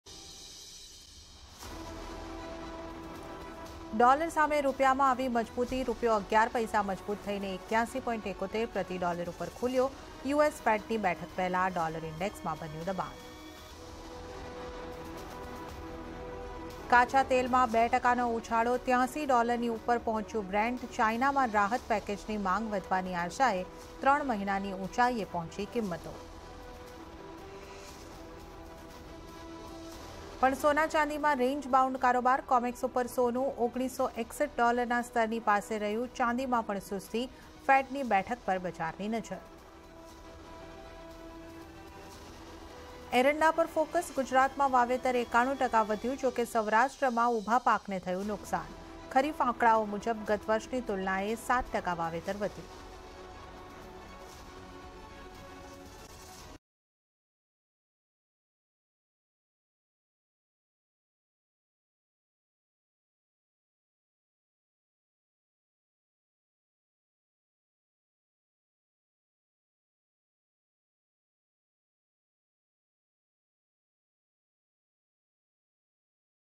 Headline Today | Commodity Market ના મુખ્ય સમાચાર | Crude | Dollar | Gold | Silver | Castor
ડૉલર સામે રૂપિયાની મજબૂતી વધી
3 મહિનાની ઉંચાઈએ ક્રૂડ
સોના-ચાંદીમાં રેન્જબાઉન્ડ કારોબાર
ગુજરાતમાં એરંડા વાવેતરમાં વધારો